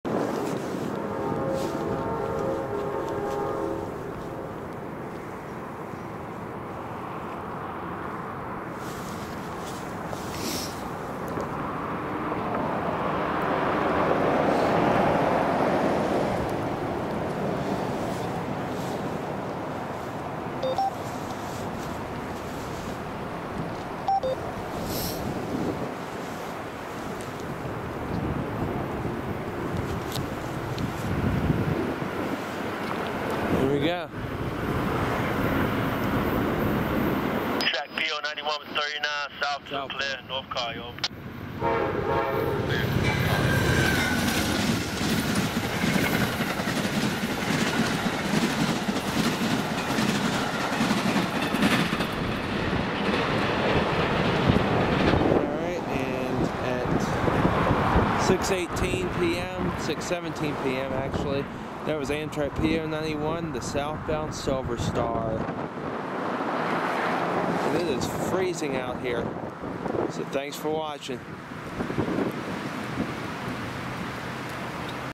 Amtrak P091
Shortly after, Amtrak P091, the Silver Star, flies south as my buddy Cory looks on. Thanks for holding on to my lights so they didn't fall this time! LOL. Shot in 4K with a Panasonic HC-VX870.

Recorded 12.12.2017